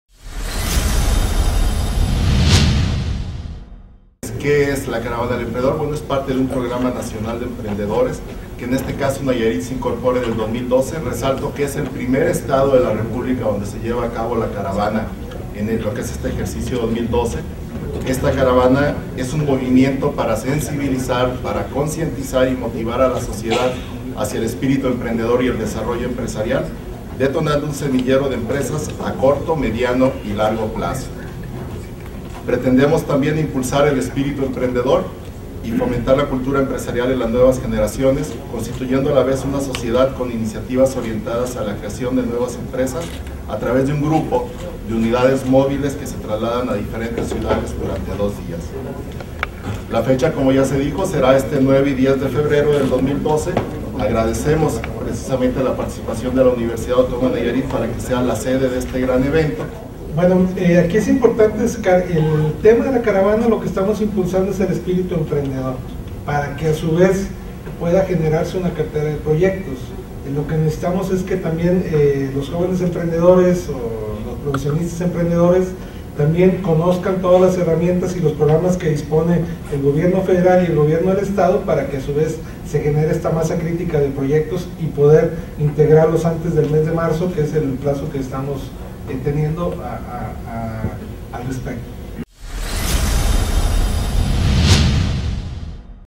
Caravana del Emprendedor Nayarit 2012.
Tepic Nayarit 07 de febrero del 2012.
Con el fin de capacitar y asesorar a todas las personas que quieran iniciar su propio negocio, este martes se presentó en rueda de prensa el  ¨Programa Caravana del Emprendedor Nayarit 2012¨.

Esta iniciativa fue presentada por importantes personalidades tales como el  Secretario de SEDECO, Alonso Villaseñor Anguiano;  Pedro Gómez Villaseñor, Delegado Federal de la Secretaria de Economía en Nayarit  y  David Acosta Cruz Secretario de extensión y vinculación UAN.

Aquí algunos fragmentos del evento donde dan más detalles de dicho programa.